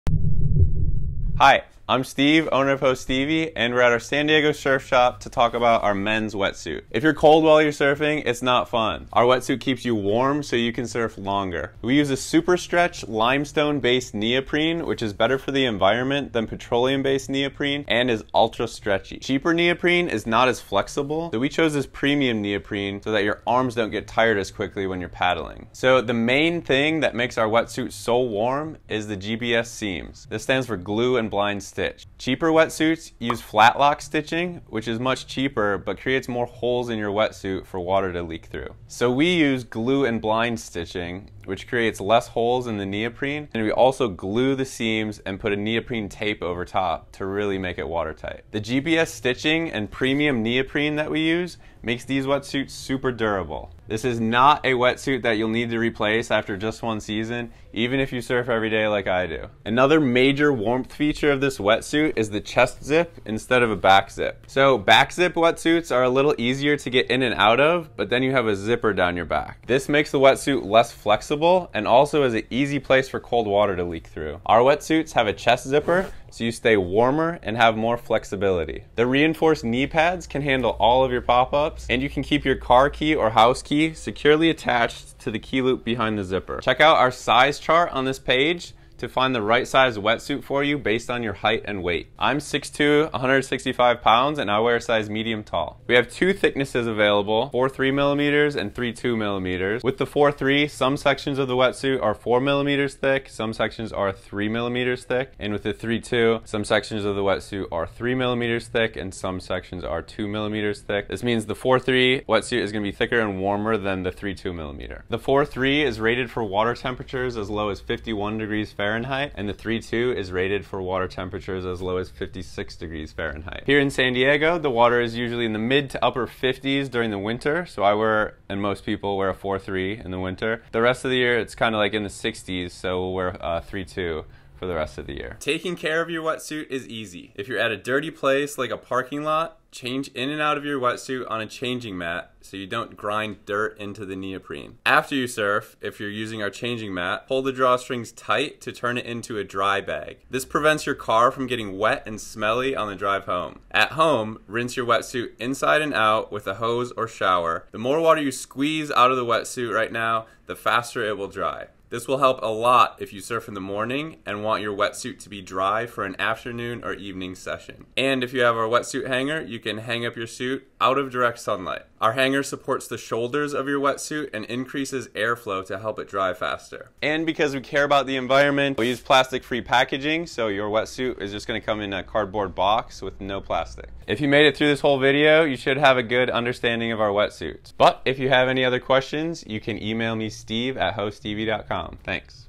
Ho Stevie! Men's Surfing Wetsuit
Our goal: provide the highest quality surfing wetsuit at the lowest price
- GBS Seams (glue and blind stitched, and taped seams)
- SuperStretch Limestone-Based Neoprene
- Chest Zip for added warmth and flexibility
- Reinforced Knee Pads
- Key Loop
- Money Back Guarantee
- Plastic Free Packaging
- Free Fast Shipping